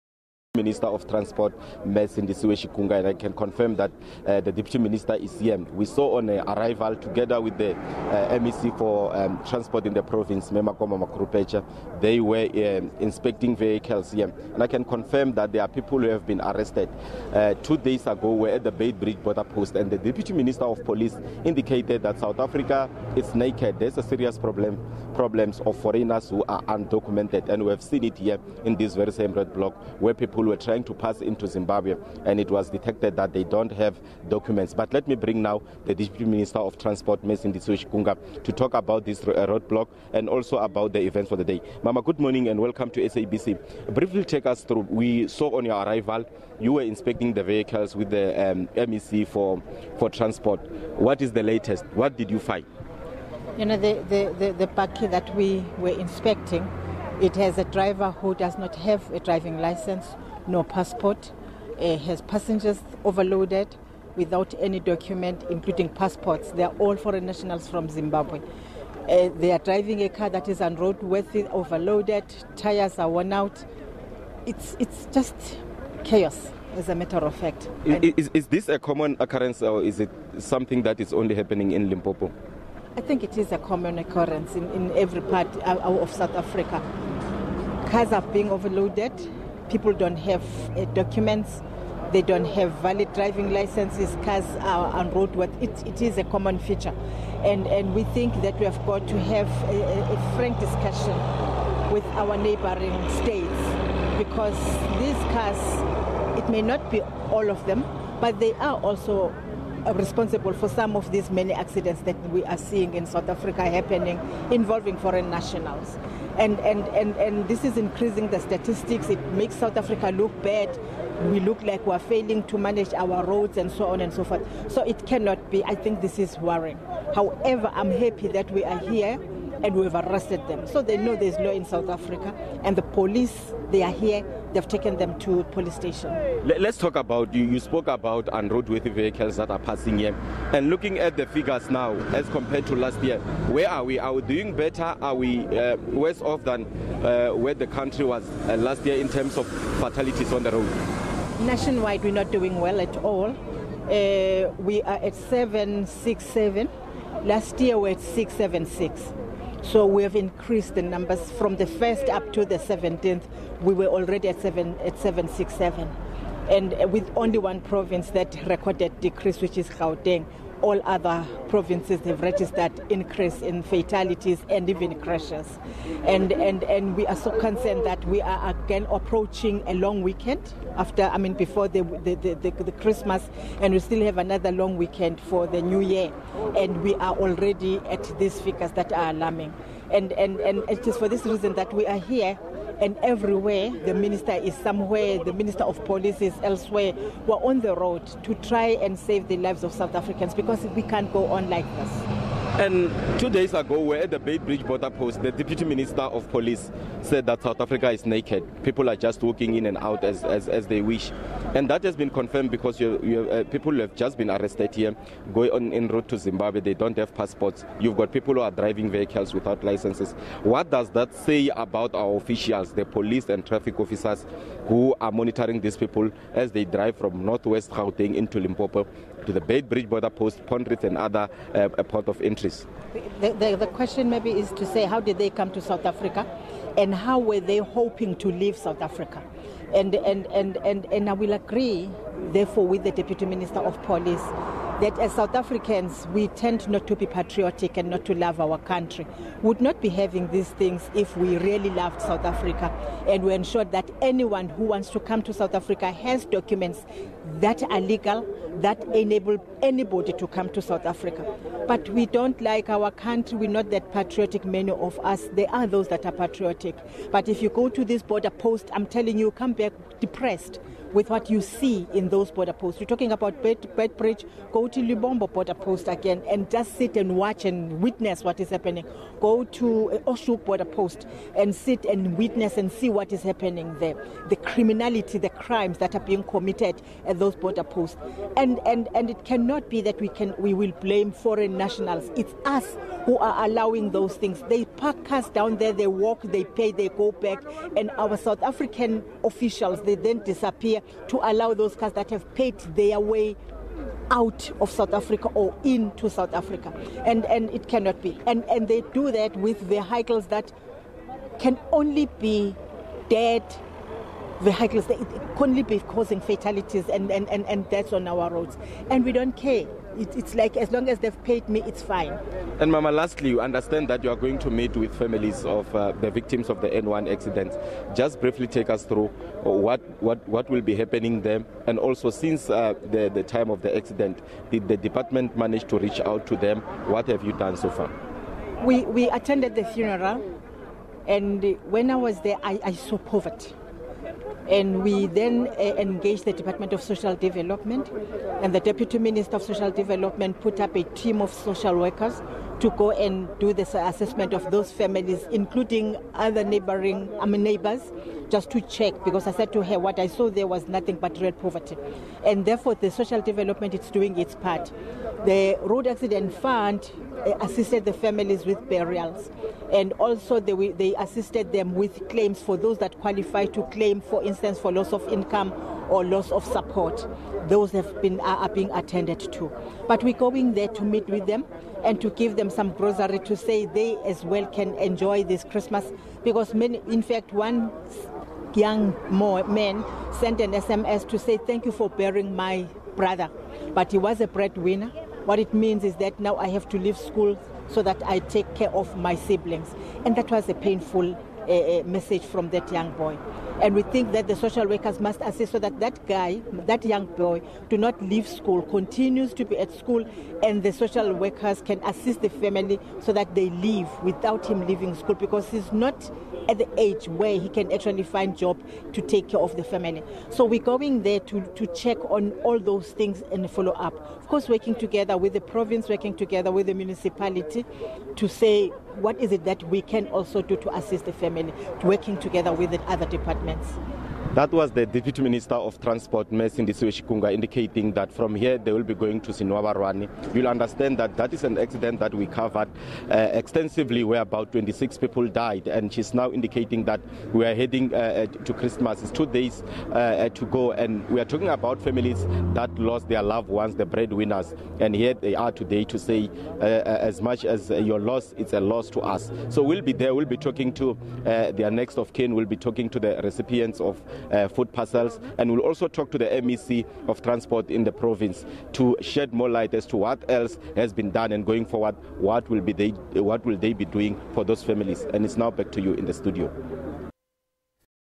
Dep. Transport Minister Chikunga cautions officials and travelers
With the festive death toll on the rise, transport authorities are pulling out all the stops to stem the tide of road crashes.  Motorists who drive drunk or reckless on the roads face major consequences. Meanwhile, the Deputy Transport Minister, Sindisiwe Chikunga is out and about on a road safety campaign in Bochum, Limpopo.